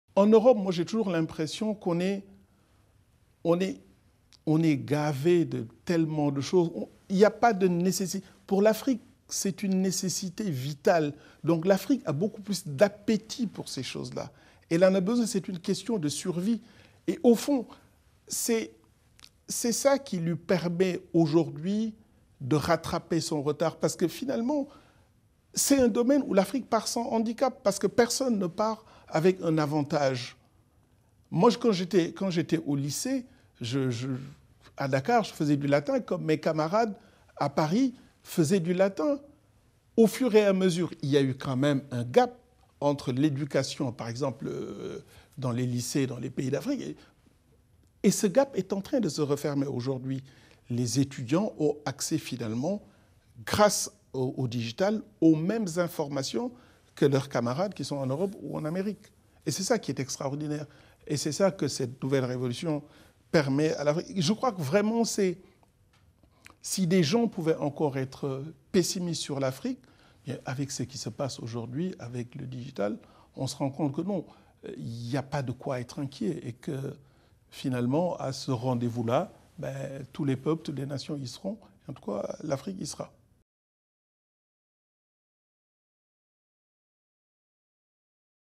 [FR] Momar Nguer : Selon vous, l’Afrique est-elle en avance sur l’Europe en matière de digital ? 4/4
Momar Nguer, Senior Vice-President Africa&Middle East du Groupe Total, a répondu à nos questions à l’occasion de la récente conférence « Cercle Premier » d’Orange Business Services consacré au rôle du digital dans le développement international des entreprises. Il revient en détail sur l’impact majeur du digital sur la croissance africaine.